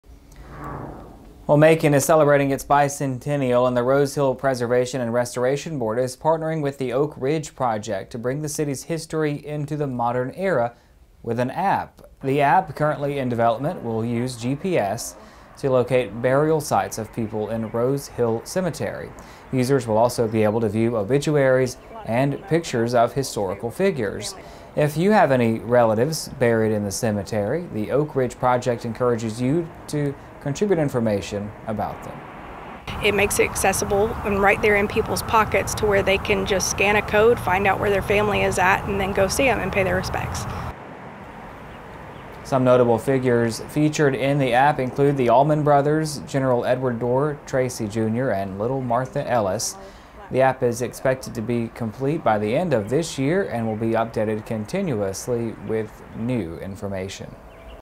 Interactive map app coming to Rose Hill Cemetery
MACON, Georgia (41NBC/WMGT) - Macon is celebrating its bicentennial, and the Rose Hill Preservation and Restoration Board is partnering with the Oak Ridge Project to bring the city's history into the modern era with an app.